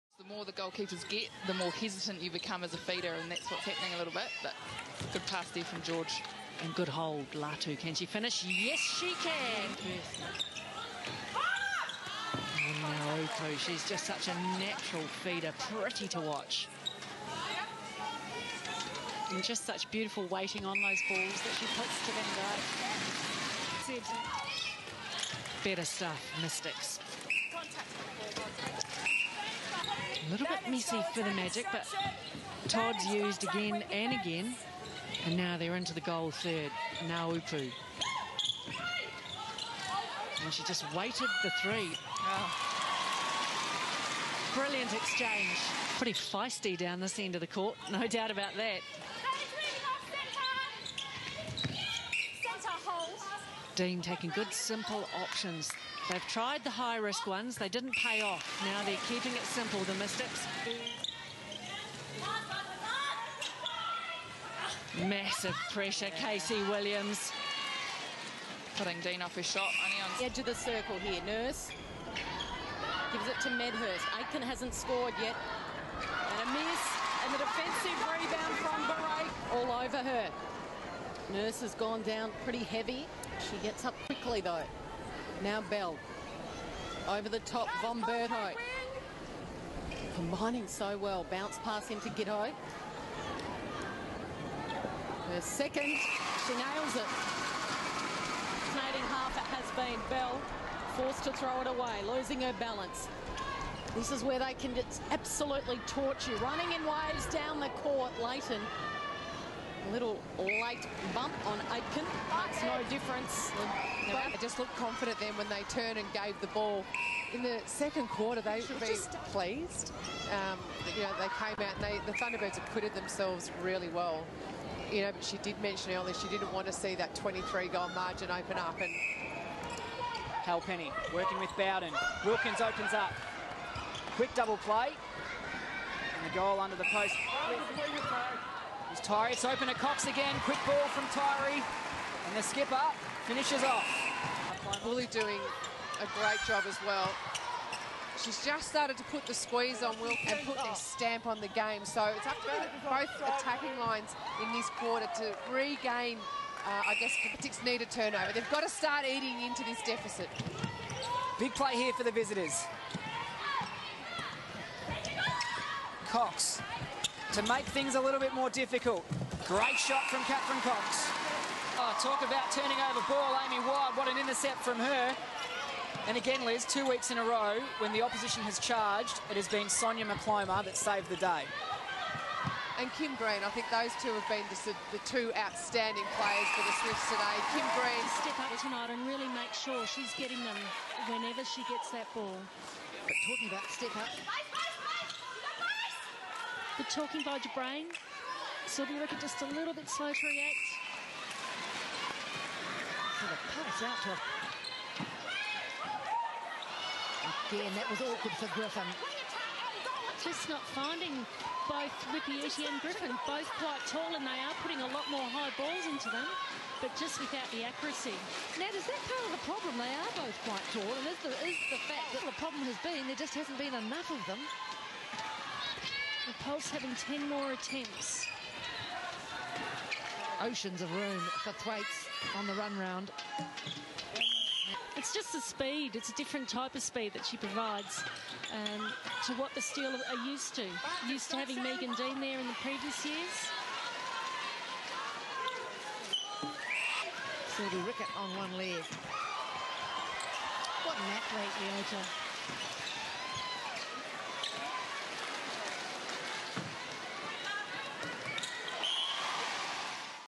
Round 5 Highlights
All the highlights from round 5 of the 2011 ANZ Championship season.